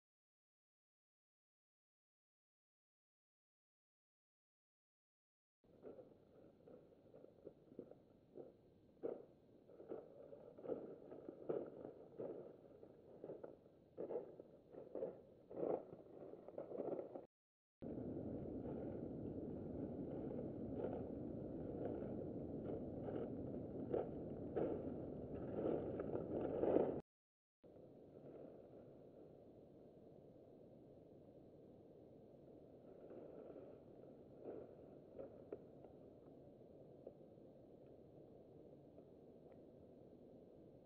Gineta cojeando en el sendero (fototrampeo)
Vemos tres tomas diferentes donde la pobre gineta tuvo algún percance en su momento.